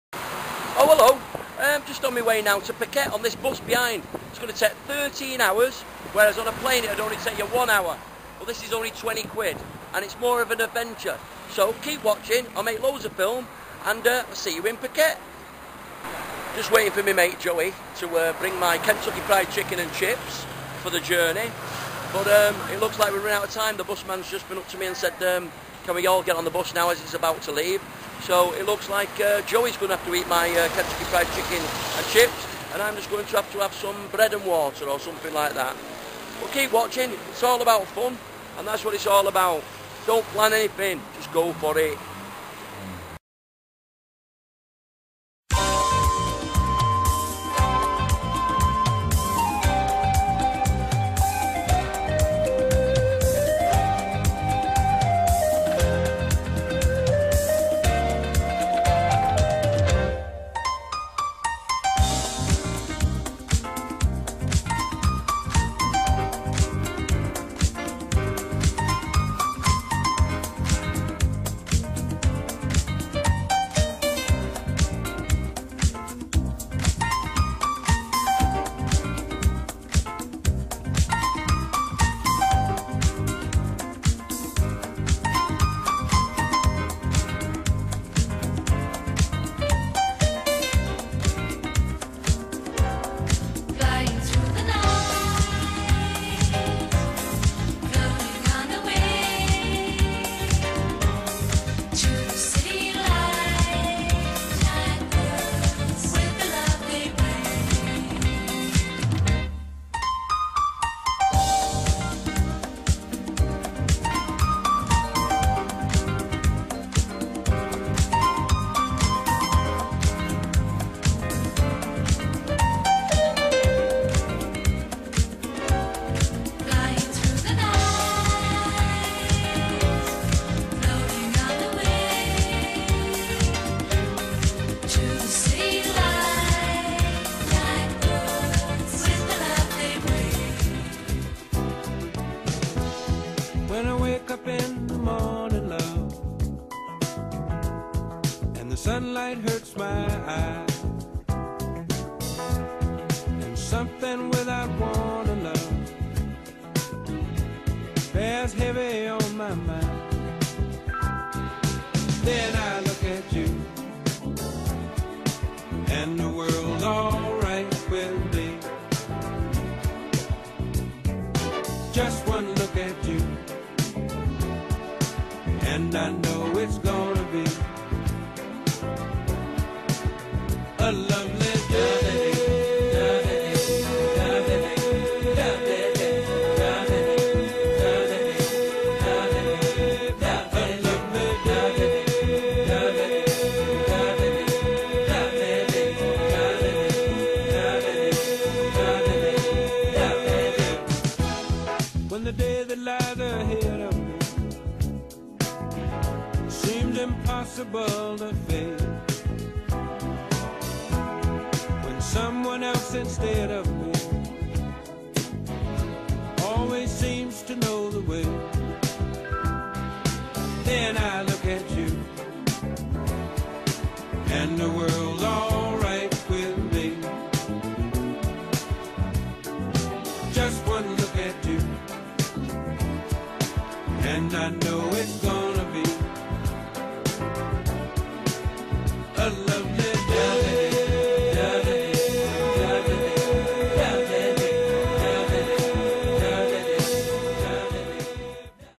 PATTAYA to PHUKET by over night bus with Geoff Carter
Save money on flights and travel by over night bus, cheap as chips !